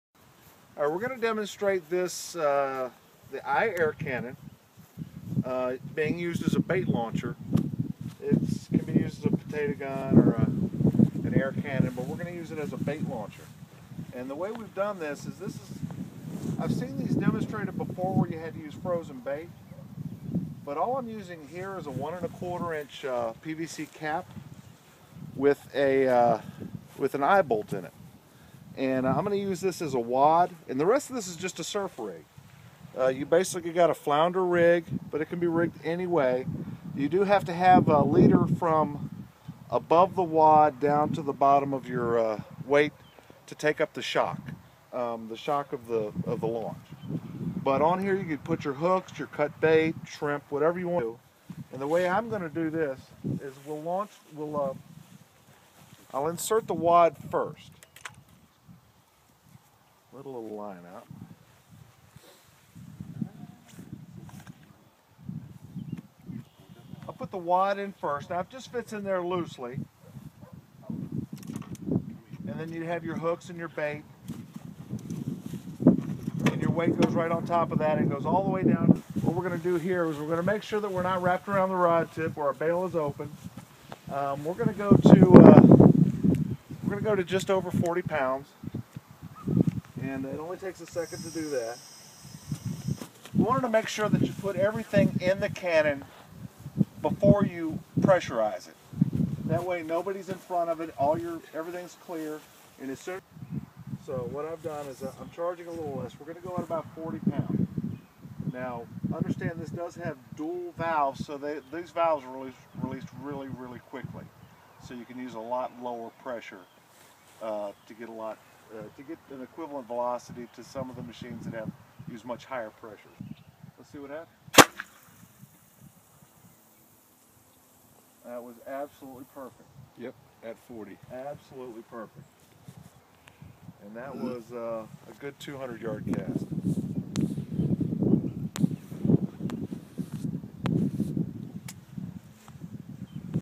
600 foot bait casting potato gun
- this is a demonstration of the new V4 air cannon launching an unfrozen surf rig over 200 yards. Features include the new 2 valve design. This design can launch projectiles over 1/4 mile.